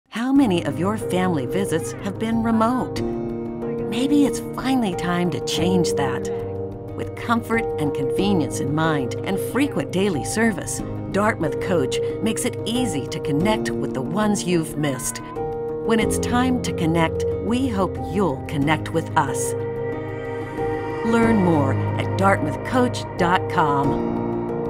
Dartmouth Coach - Time to Connect
How many of your family visits have been remote? Maybe it's finally time to change that. Dartmouth Coach makes it easy to connect with the ones you've missed.